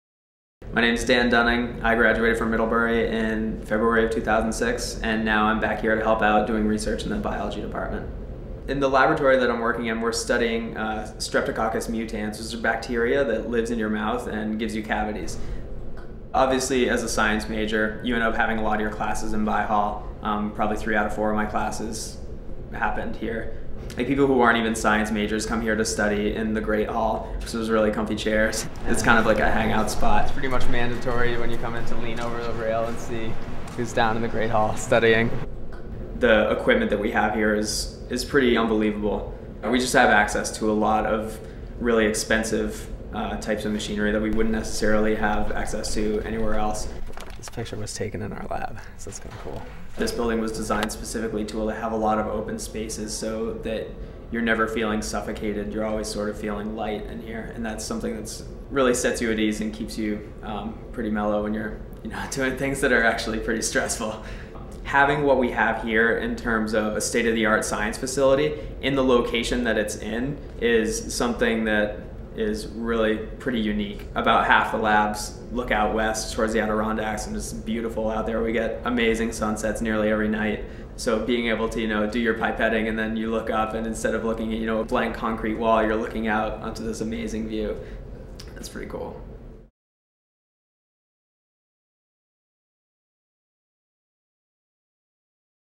Visit the Science Center
Tour McCardell-Bicentennial Hall, Middlebury College's science center, which houses classrooms, faculty offices, labs and a science library, and has become a popular meeting place for students